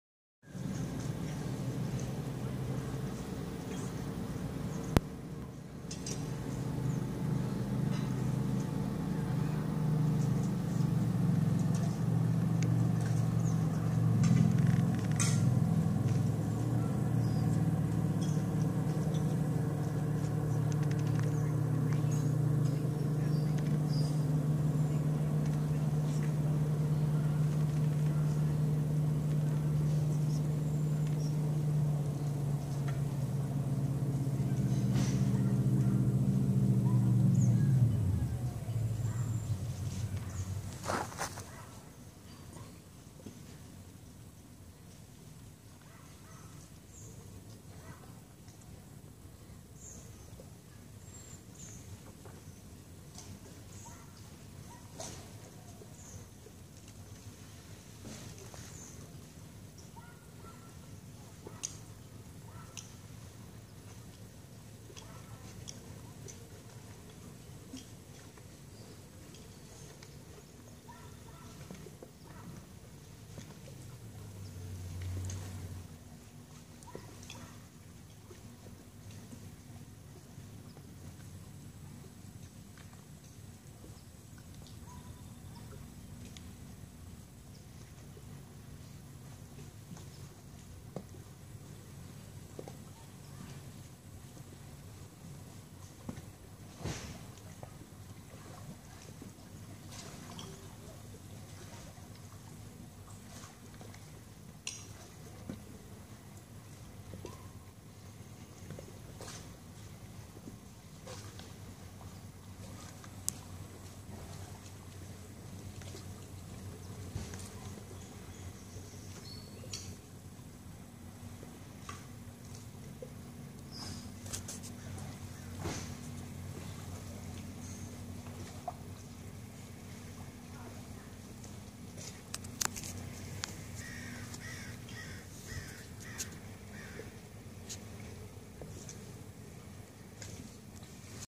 Ants dragging caterpillar to god knows where
2 ants dragging a live caterpillar back to the hill